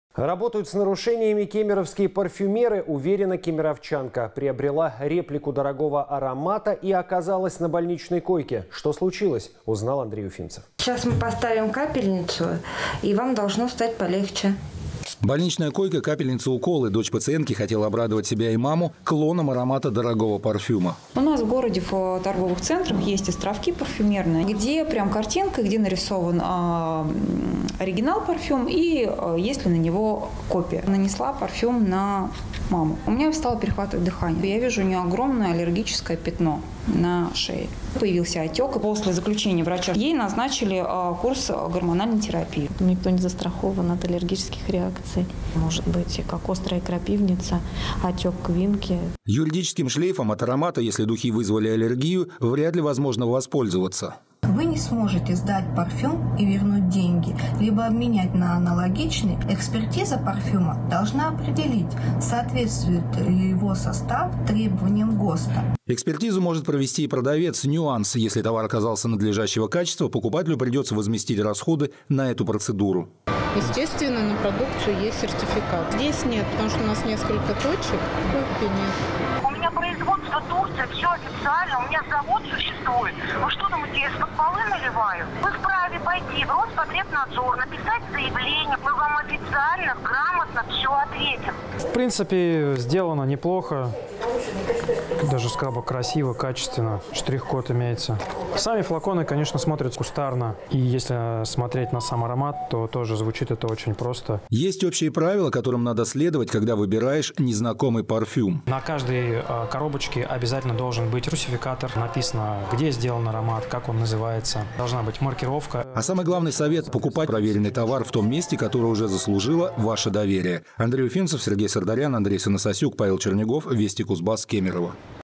Покупка «реплики» дорогих духов довела кемеровчанку до больничной палаты
Покупка «реплики» дорогих духов довела кемеровчанку до больничной палаты . Друзья ,всем привет  lavparfum cнова на  тв .Как думаете может такое быть ?